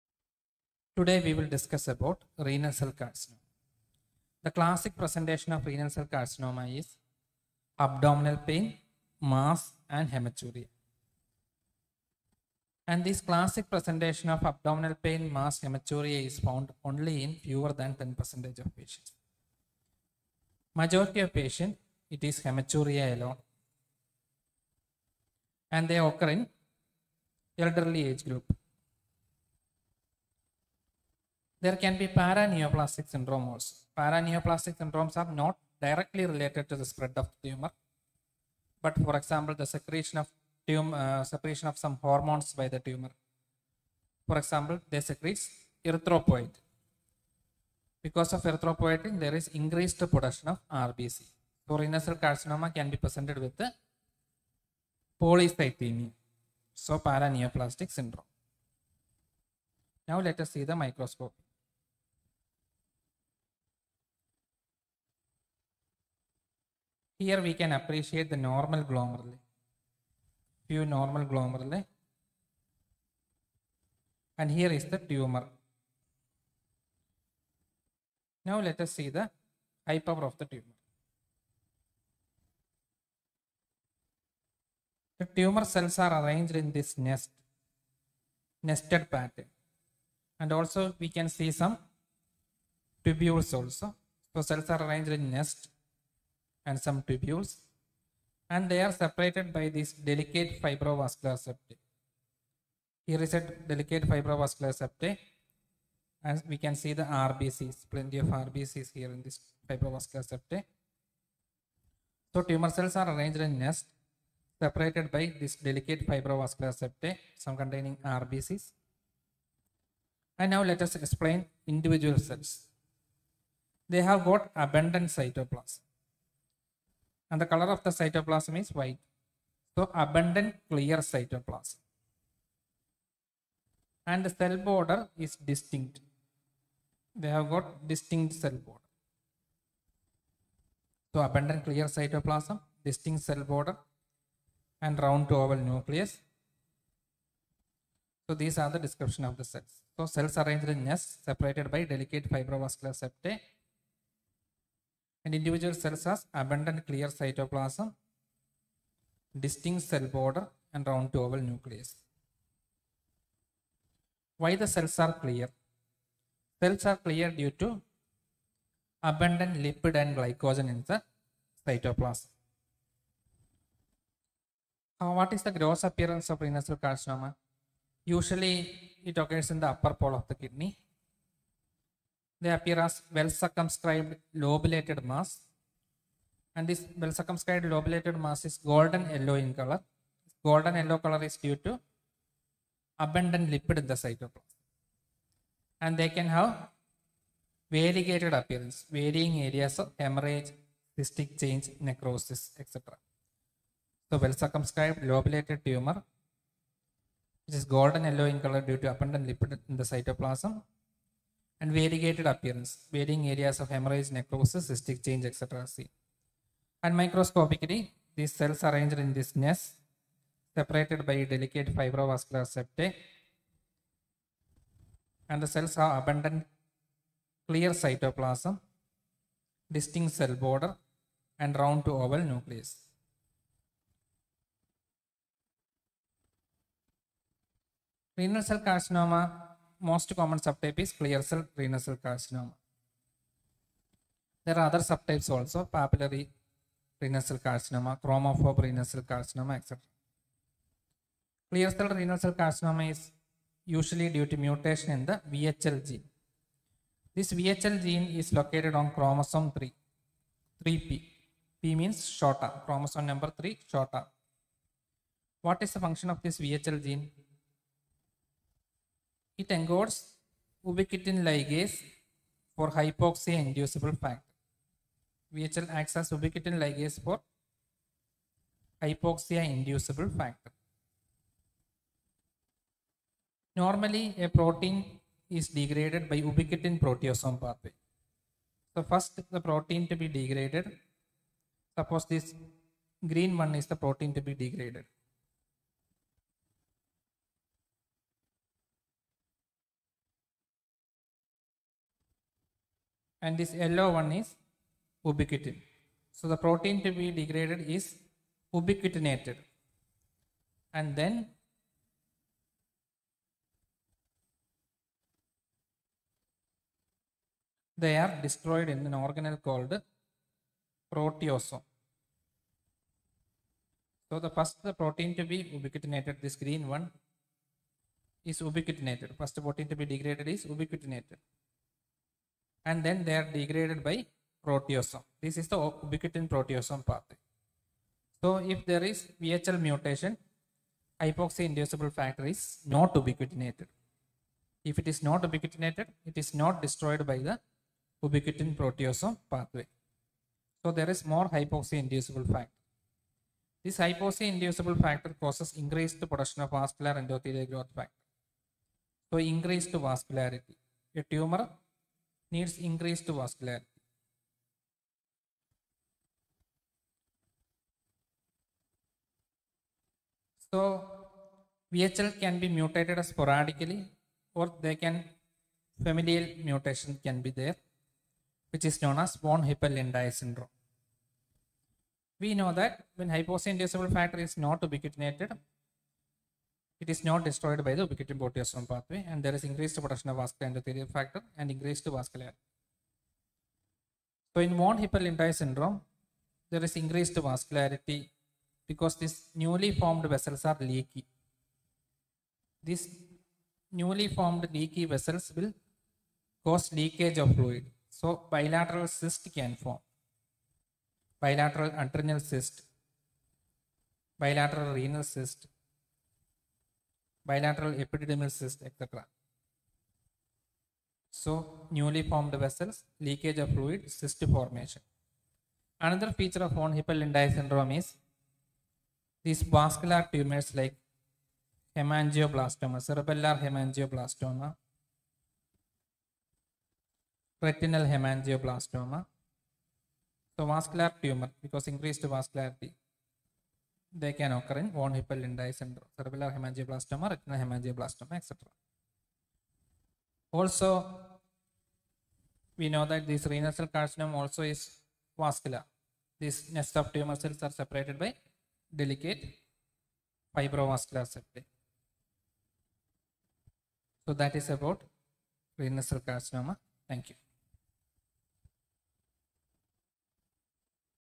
clear cell renal cell carcinoma
Discusses about microscopy of clear cell renal cell carcinoma